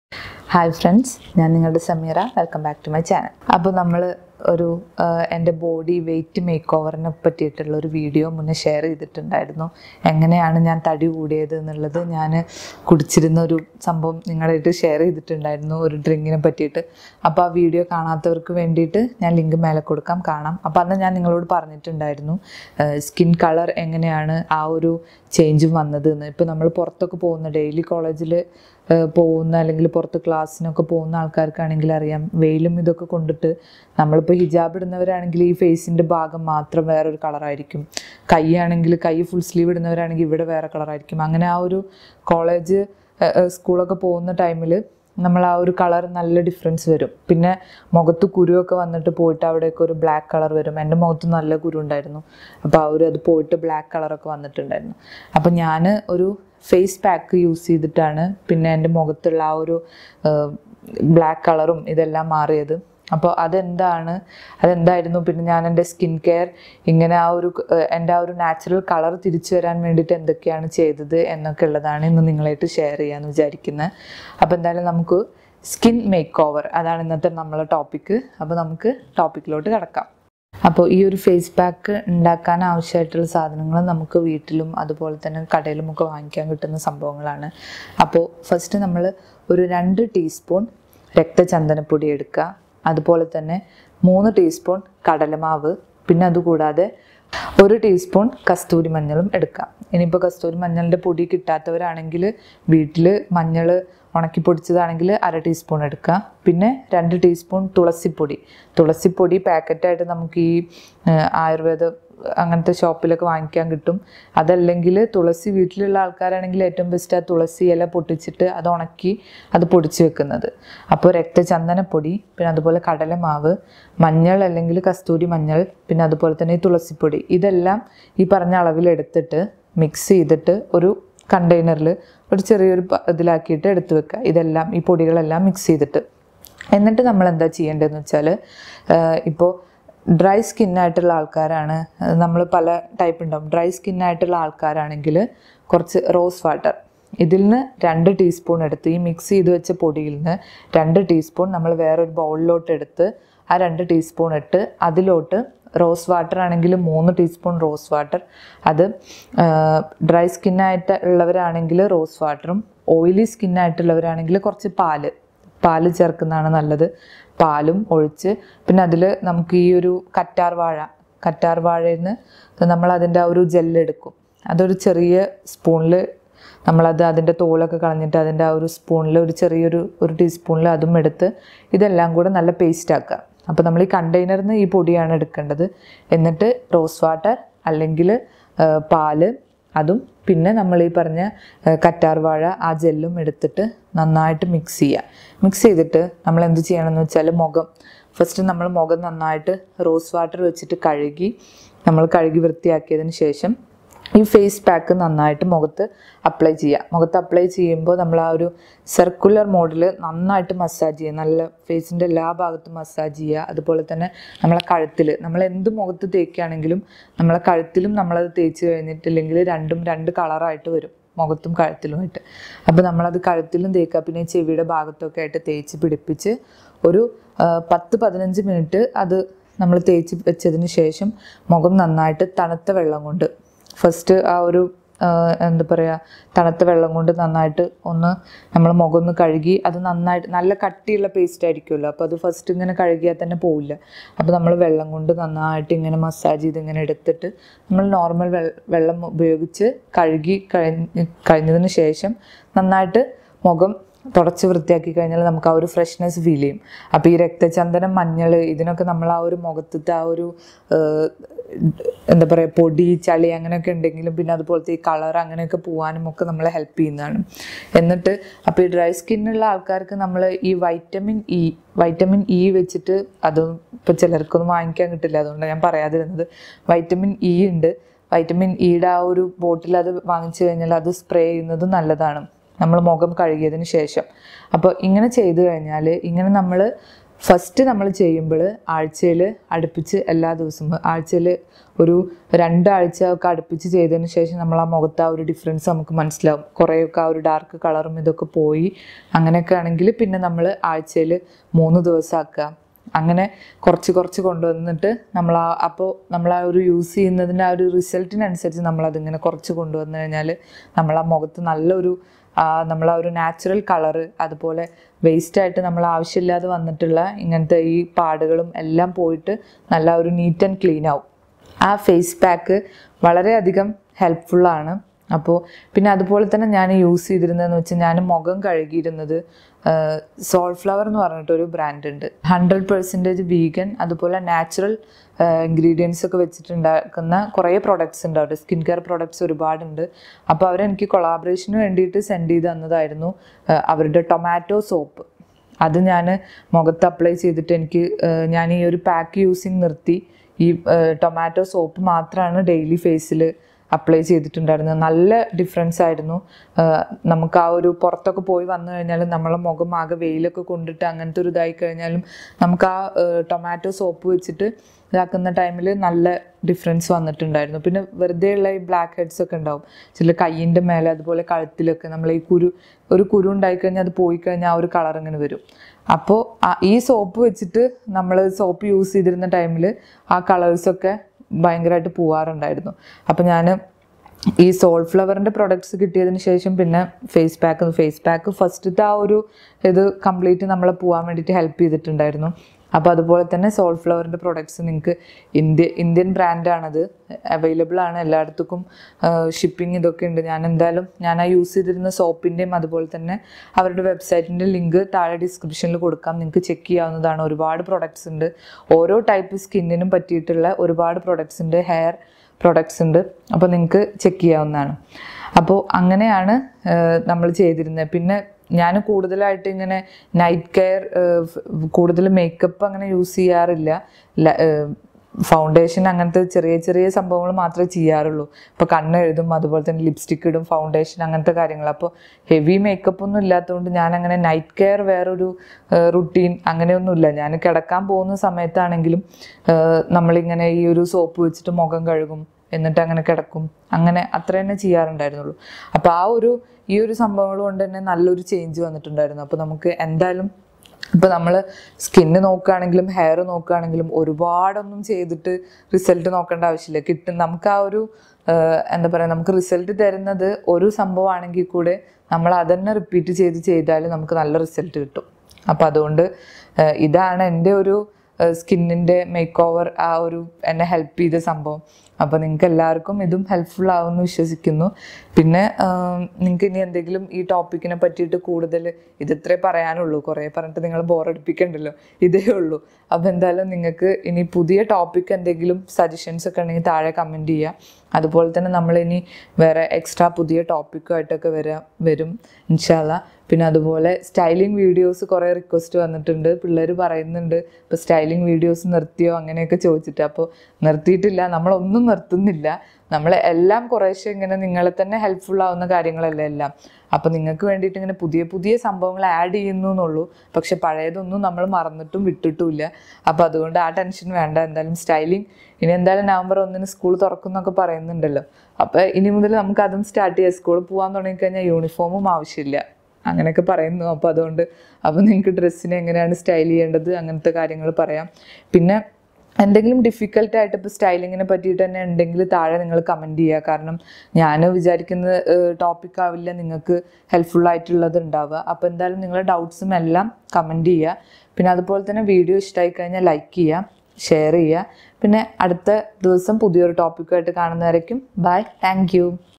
MY SKIN MAKEOVER STORY | ഈ നാല് ചേരുവകൾ മതി മുഖം തിളങ്ങാൻ!!!| STYLE WITH SAMEERA
ഹായ്,

ഞാനിന്ന് നിങ്ങളുടെ മുന്നിലെത്തിയിട്ടുള്ളത് എന്റെ സ്കിൻ മെയ്ക് ഓവർ സ്റ്റോറി നിങ്ങളുമായി ഷെയർ ചെയ്യുവാനാണ്. ഞാൻ നിത്യേന ഉപയോഗിച്ചിരുന്ന ഫേസ് പാക്കിന്റെ ചേരുവകൾ താഴെ കൊടുക്കുന്നു. നിങ്ങളുടെ വിലയേറിയ അഭിപ്രായങ്ങളും നിർദേശങ്ങളും താഴെ കൊടുക്കുക.

രണ്ട് ടീസ്പൂൺ രക്തചന്ദനപ്പൊടി
മൂന്ന് ടീസ്പൂൺ കടലമാവ്
ഒരു ടീസ്പൂൺ കസ്തൂരി മഞ്ഞൾ അല്ലെങ്കിൽ അര ടീസ്പൂൺ മഞ്ഞൾ പൊടി
രണ്ട് ടീസ്പൂൺ തുളസിപ്പൊടി